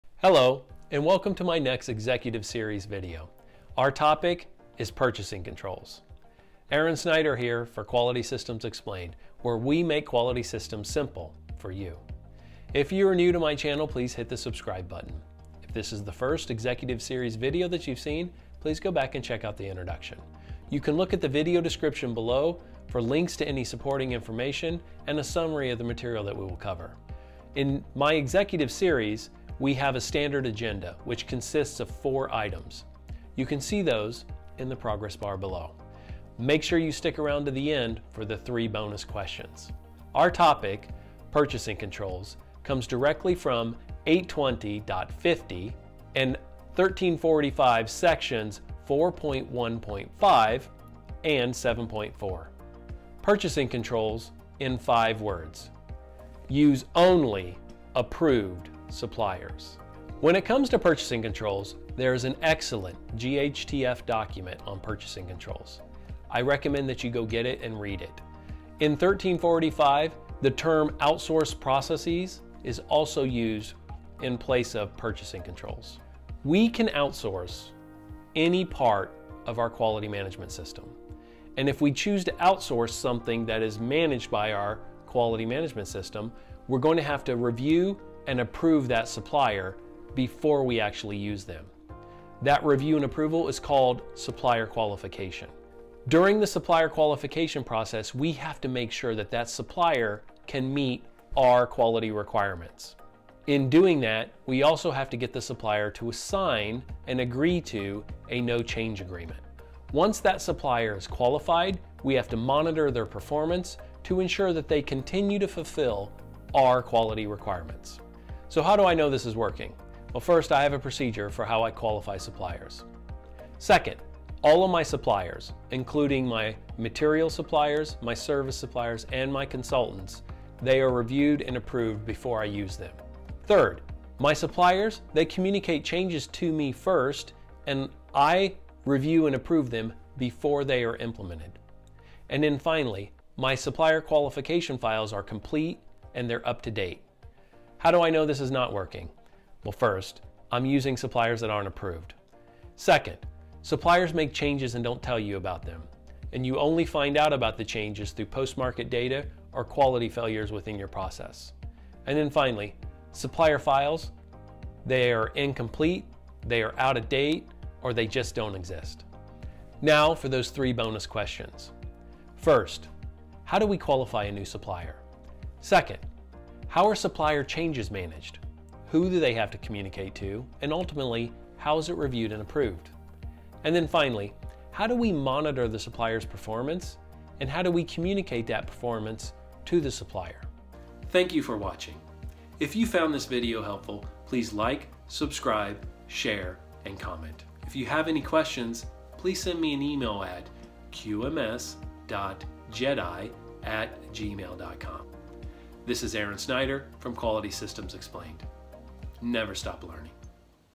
Purchasing Controls 820.50 & ISO 13485 § 4.1.5 & 7.4 (Executive Series #28)
o Chapter 1
              Task 5
      o Chapter 3
              Task 13
      o Chapter 7
              Tasks 1, 2, 3, 4, 5, 6, 7, 8, 9, 10, 11, and 12
      o Annex 4

Quality Management System – Medical Devices – Guidance on the Control of Products and Services Obtained from Suppliers, By GHTF

FDA Small Business Regulatory Education for Industry (REdI) Conference, by Aileen I. Velez Cabassa

Our requirement, Purchasing Controls, comes directly from 820.50 and 13485 Section 4.1.5 & 7.4

Purchasing Controls in 5 words:
Use Only Approved Suppliers